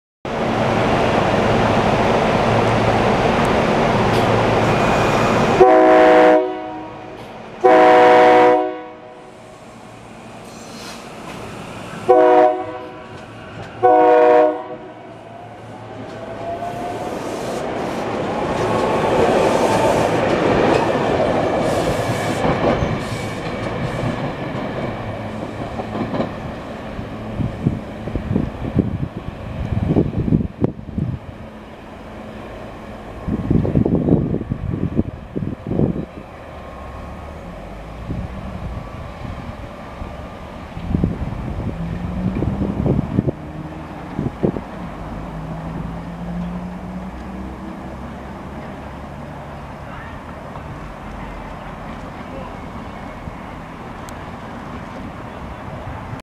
SEPTA Regional Rail Train #8360 Departing Langhorne, PA
SEPTA Regional Rail Train #8360 departing from the station at Langhorne, Pennsylvania & blowing for the road crossing running 7 minutes late bound for West Trenton, NJ across the river with a 4-Car set of Rotem Silverliner V's. I then packed it up shortly afterwards.